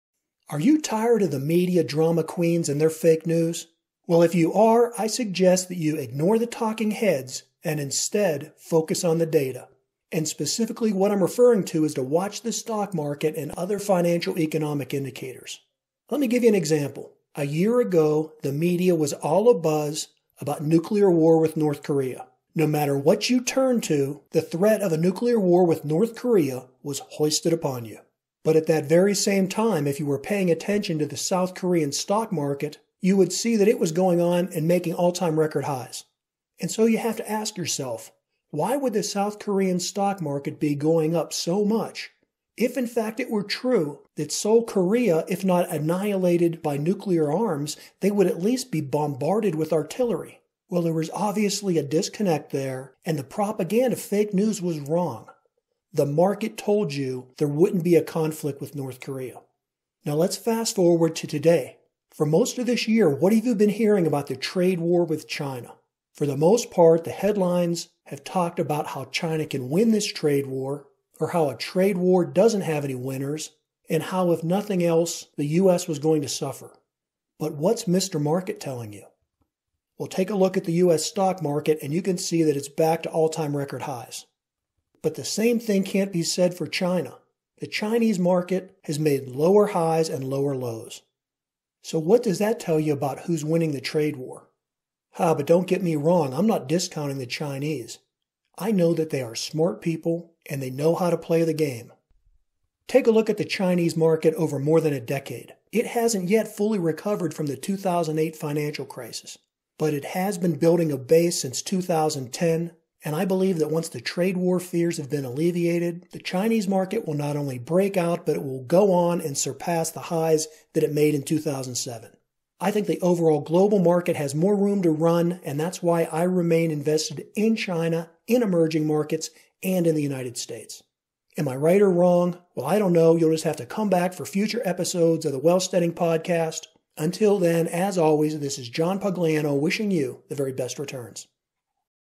Listen to the market
Tired of the media drama queens and fake news?

Ignore what’s being said and instead focus on the data.  Especially watch the Stock & Financial Markets.  Rather than taking the Red Pill to escape the Matrix, listen to the Market and profit from it.

Listen to the full audio version of the Wealthsteading Podcast on your favorite syndicator or directly